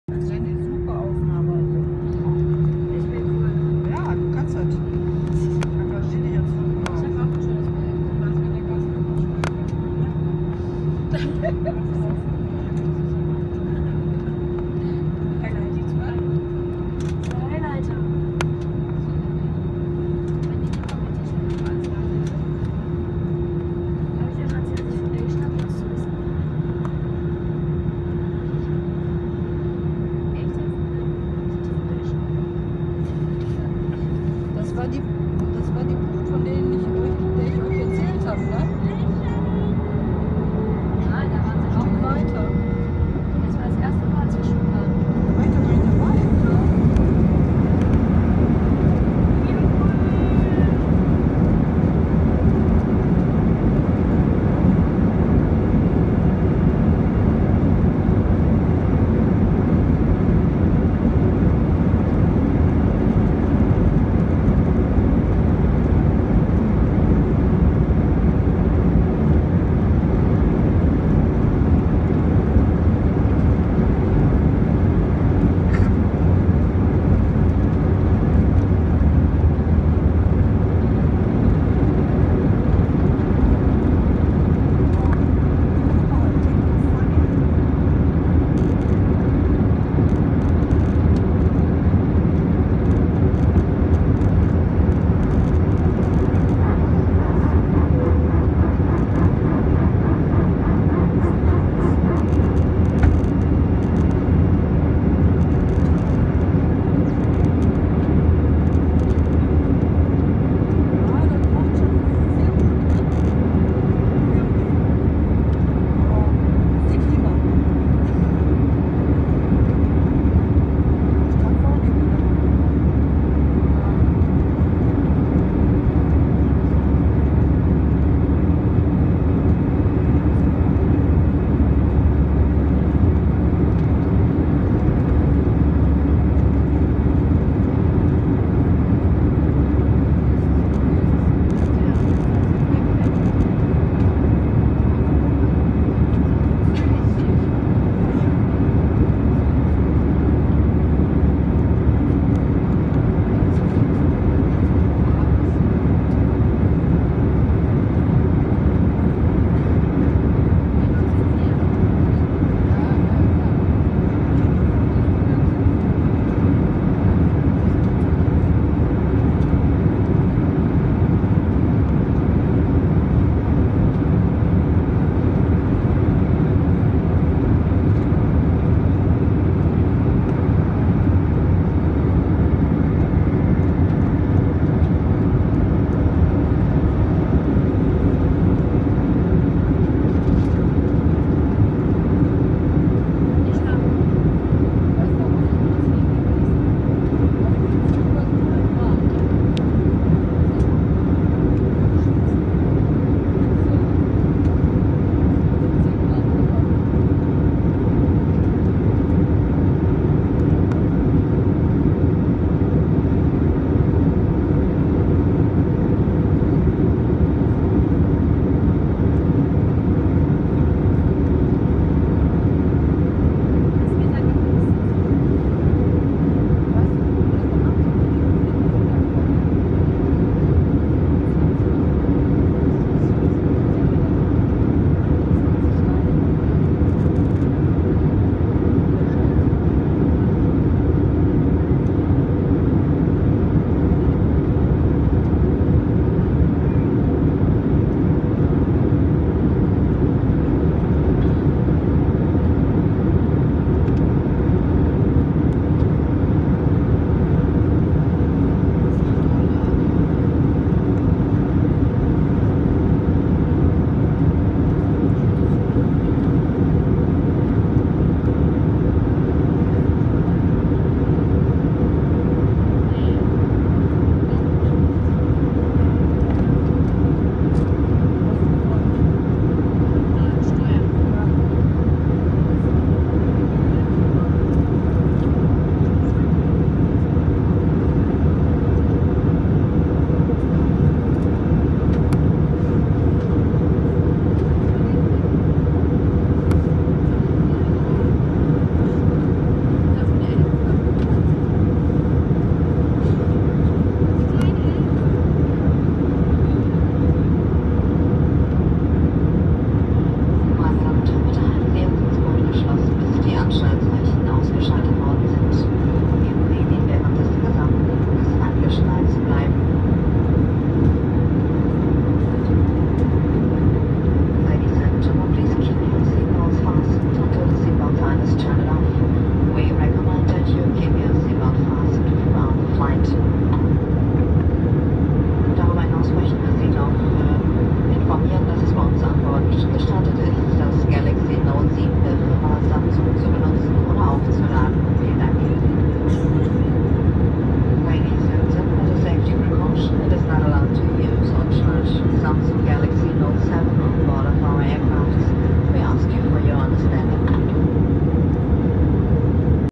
TakeOff Sant Jordi de ses Salines mit AirBerlin AB5379
Tschüss Ibiza- Hallo Düsseldorf! Kurztrip nach Ibiza zur besten Küche der ganzen Insel. Am Vedrá Felsen bei Cala d’ Hort bei wunderschönem Sonnenuntergang hatten wir einen fantastischen Abend mit meinen Freunden.